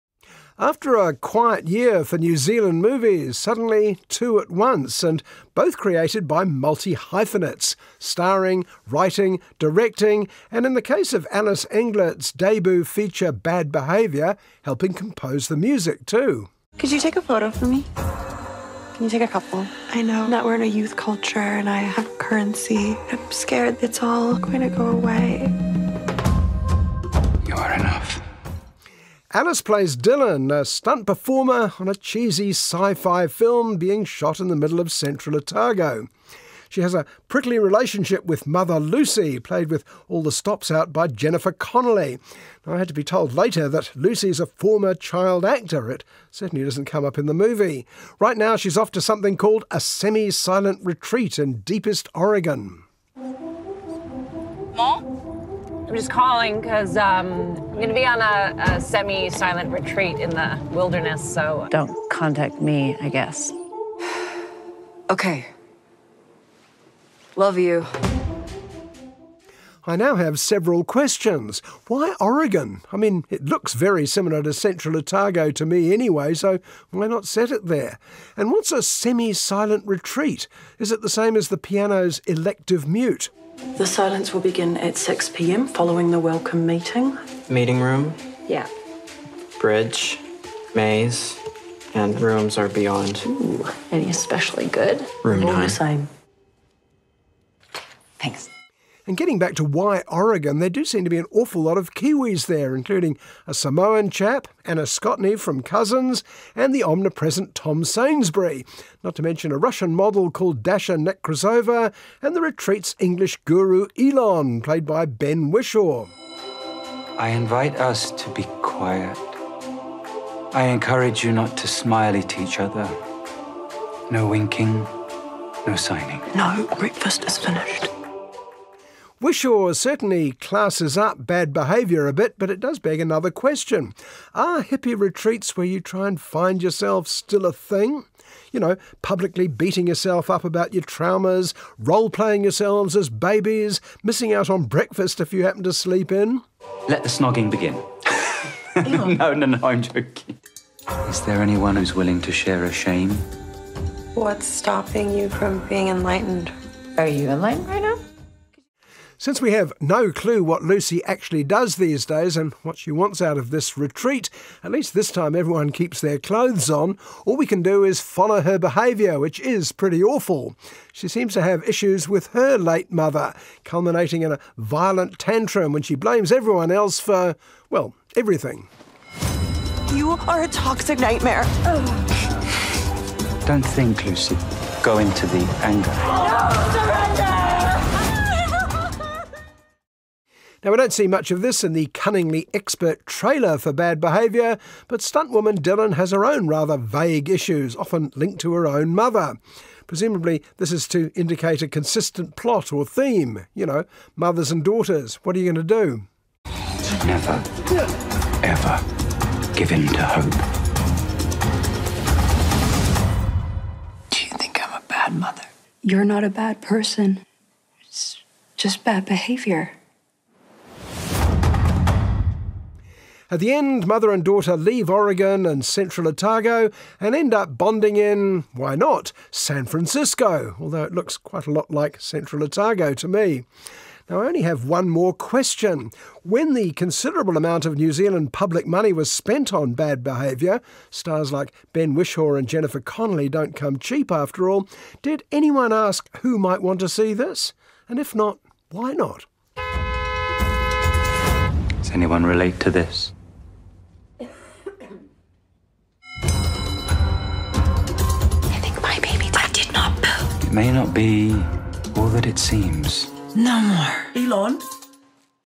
Review: Bad behaviour | At The Movies | RNZ
Bad Behaviour, the debut feature by New Zealand writer-director Alice Englert, explores the mother-daughter relationship of a movie stunt woman and a former actress in search of enlightenment. Stars Jennifer Connolly (A Beautiful Mind) and Ben Wishaw (Skyfall).